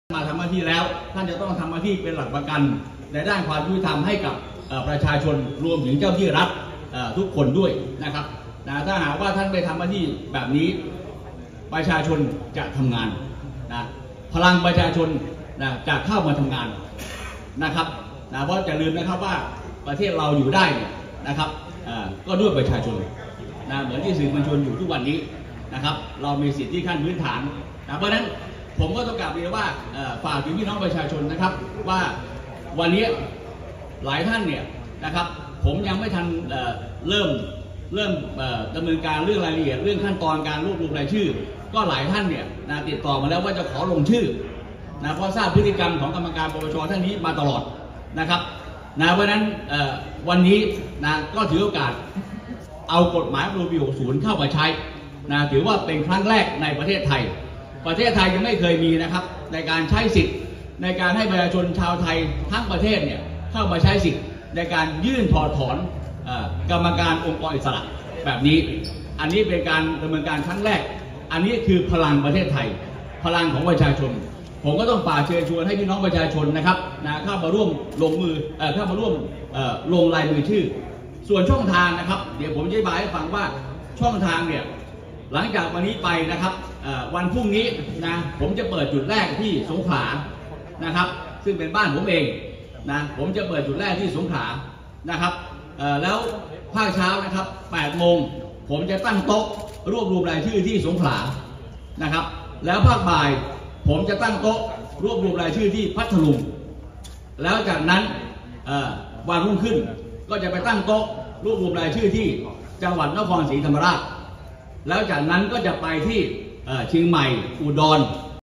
อ้างปรากฏการณ์พลังประชาชน "โจ๊ก" โร่ยื่น "วันนอร์" ล่าชื่อถอดถอนกรรมการ ป.ป.ช.
“บิ๊กโจ๊ก” บุกสภา ยื่น“วันนอร์” ล่าชื่อถอดกรรมการ ป.ป.ช.ฐานมีพฤติกรรมส่อทุจริต มั่นใจ 2 สัปดาห์ได้ครบ 2 หมื่นชื่อ อ้างหรูเป็นปรากฏการณ์ใหม่ แสดงออกถึงพลังประชาชนเป็นครั้งแรกในประวัติศาสตร์